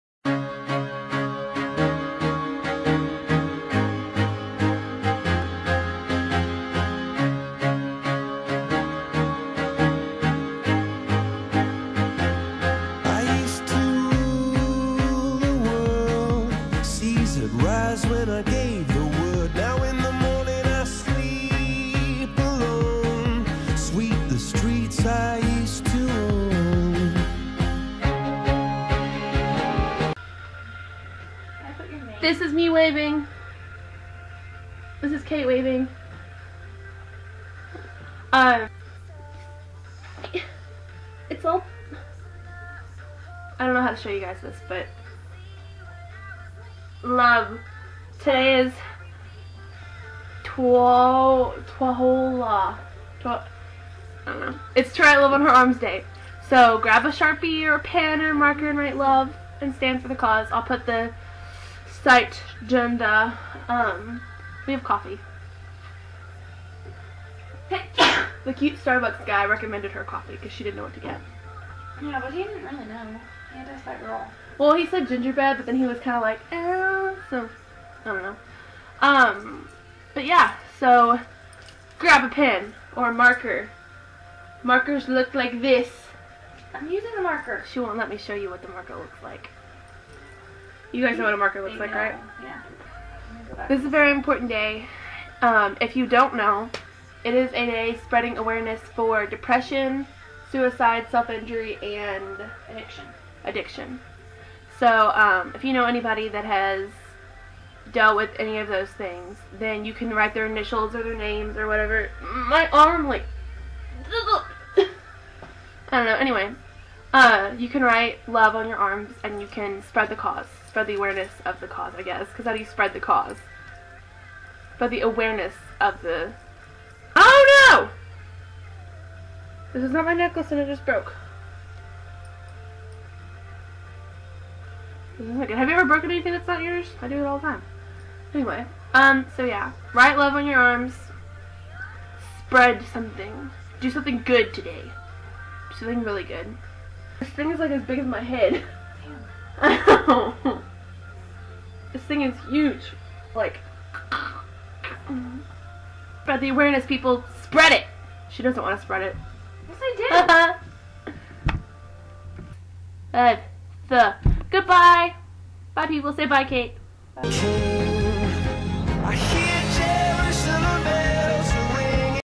You Can Make A Difference - TWLOHA
This day is a great day to spread awareness of suicide, self-injury, addiction, and depression. Its easy. Just grab a marker, write Love on your arm, or just your wrist, and when people ask, tell them about TWLOHA!!(: